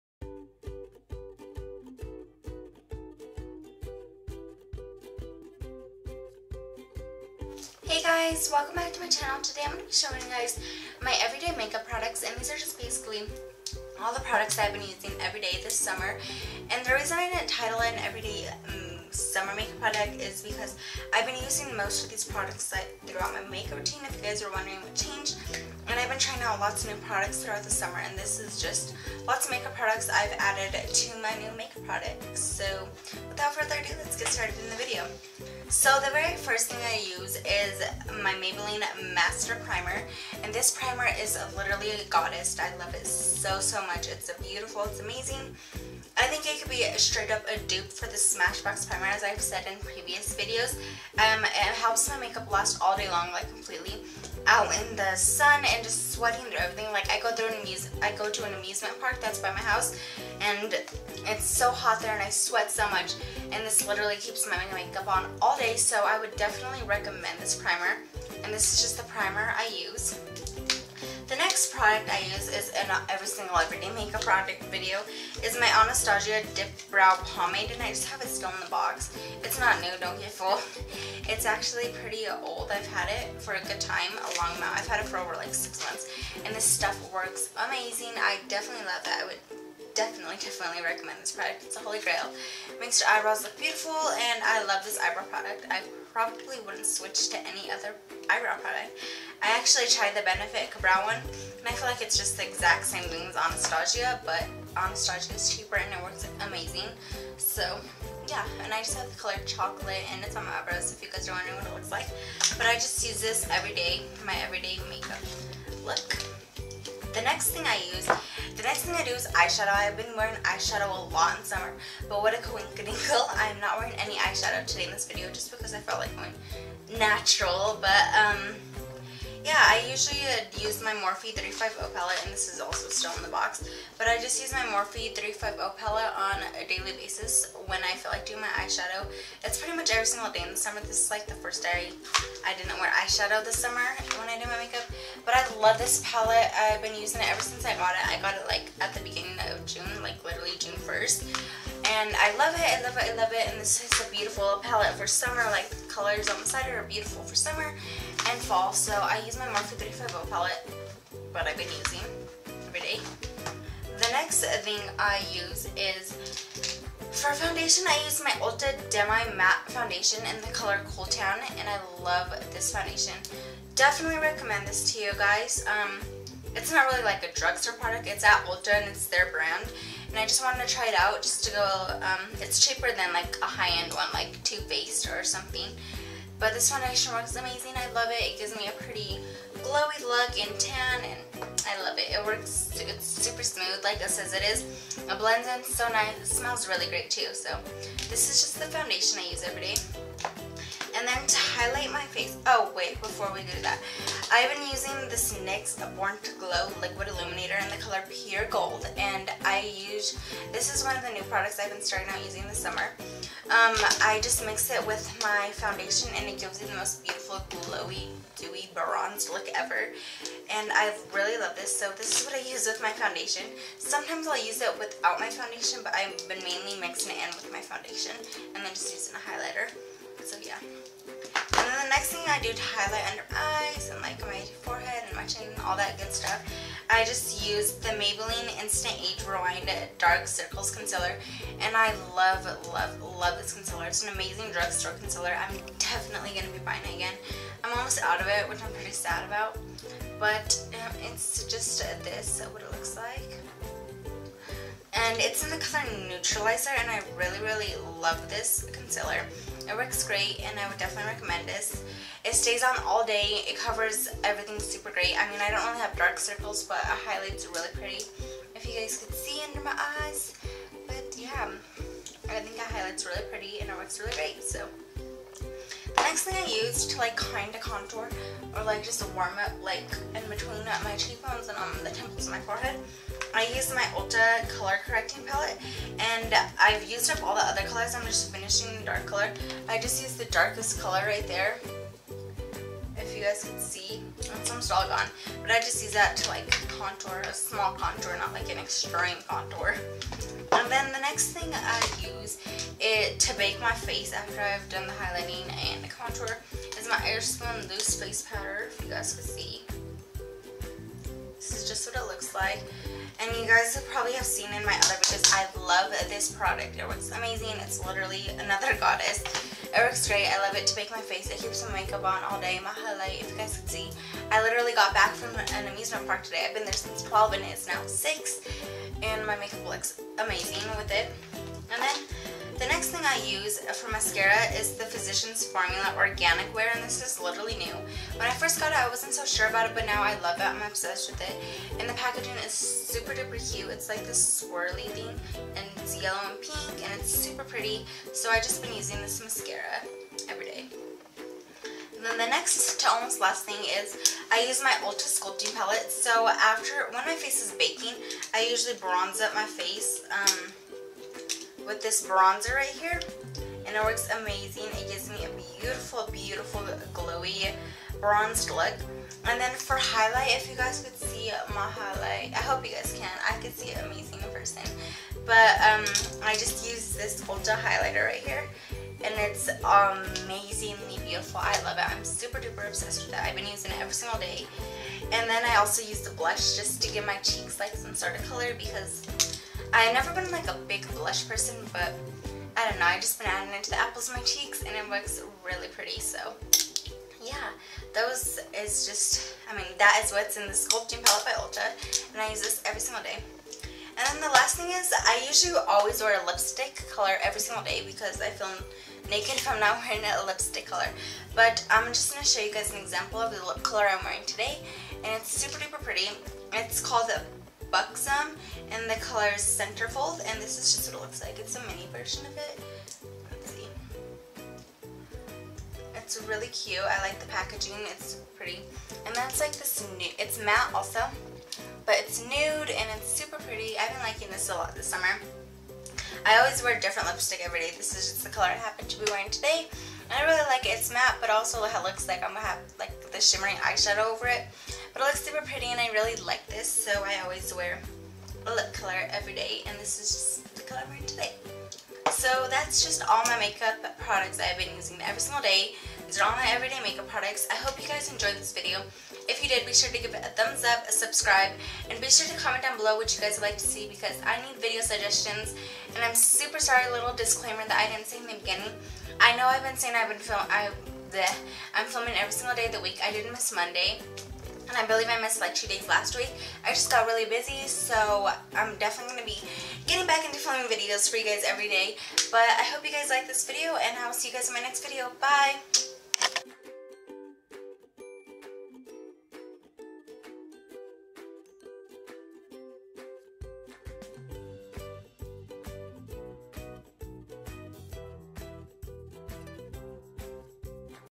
Everyday Makeup Products
hey guys i hope you enjoyed this!
theres details about products bellow

Primer: Maybaline Master Primer
Eyebrows: Anastasia Dip brow pomade
Eye shadow Pallete: Morphe 350
Foundation: Ulta demi matte foundation
Concelear: Maybaline instant age rewind concelear
countour: ulta color correct pallete
baking: airspun loose face powder
bronzer: ulta sculpting pallete
highlighter:ulta
illuminator: nyx born to glow liquid illuminator
lips: buxom lip matte